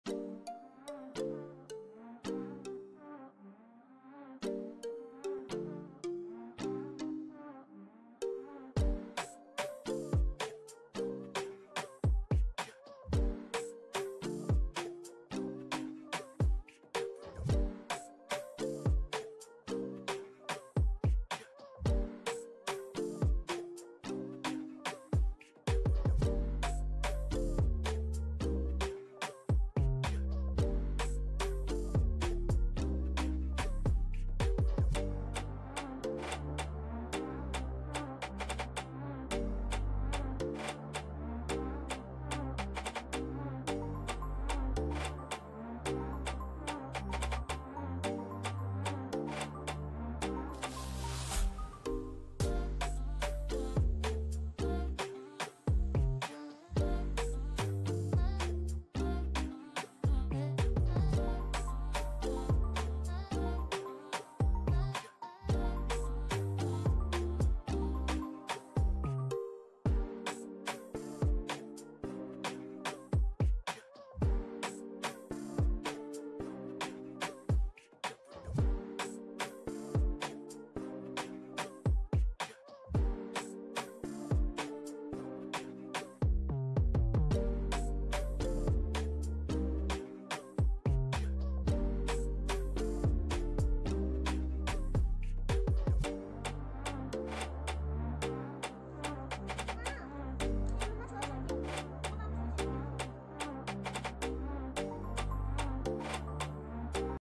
Veg spring roll with homemade sheets| Vegetables Spring roll| Swad ka tadka 😋
Spring roll recipe in Hindi, wrapper recipe in hindi.

Ingredients-
Maida
Refined oil
Water
Black pepper
Red chillies powder
Salt
Onion
Capcicum
Cabbage
Carrot
Peas
Maida paste
Soya sauce
Tomato sauce
Chilli sauce
Vinegar.